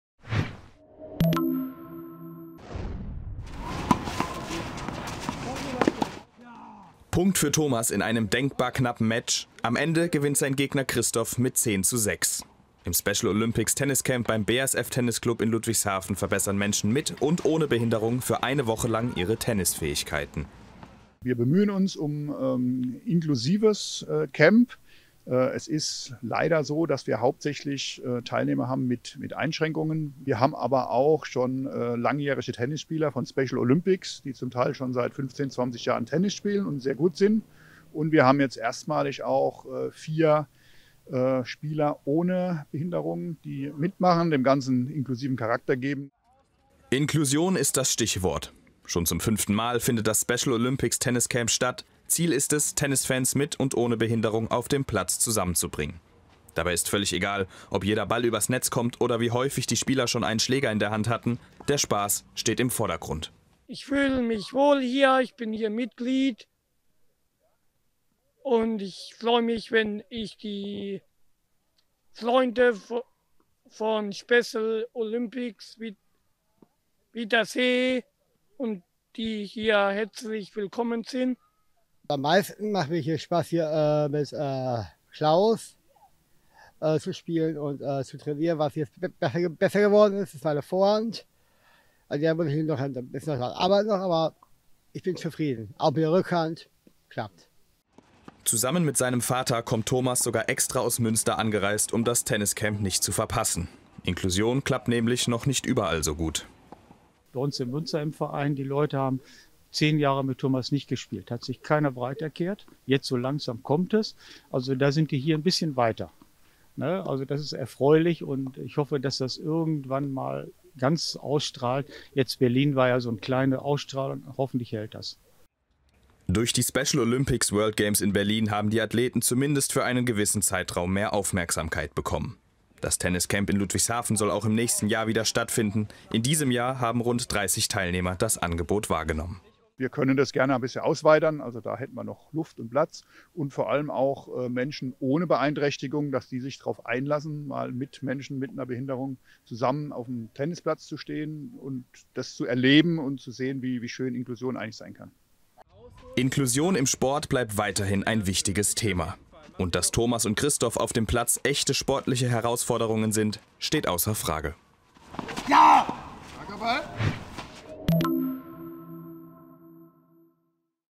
Das BASF-Tenniscamp bietet Tennis für jeden an | RON TV
Tennis für Inklusion: Special Olympics, also Sport für Menschen mit körperlichen und geistigen Behinderungen, sind spätestens seit den World Games in Berlin in diesem Jahr ein großes Thema. Aber auch Inklusion, also nicht nur Sport FÜR sondern auch MIT behinderten Menschen wird immer präsenter. Genau dafür gibt es beim BASF-Tennisclub in Ludwigshafen ein einwöchiges Tenniscamp – wir haben uns das Ganze mal angeschaut.